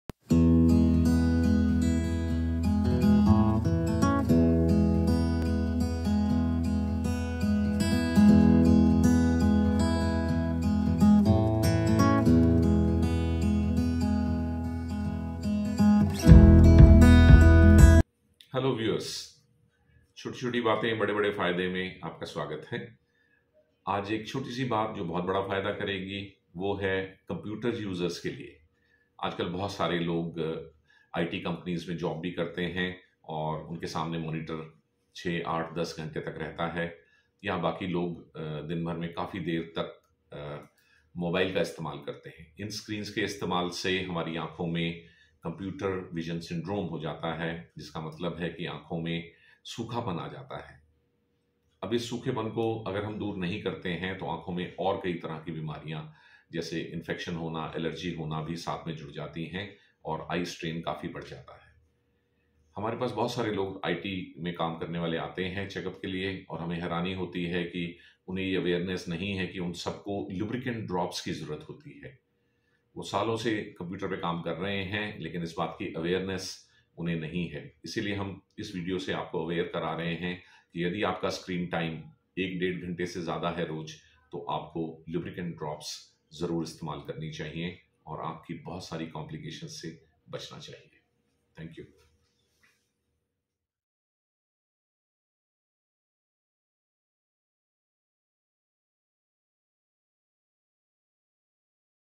Computer Users- Important Tips.
Does using computer affect your eyesight?
How do computer users care for their eyes?
Surya eye care centre (Dr. Rakesh Joshi )
10/150 Geeta Colony
Delhi -110031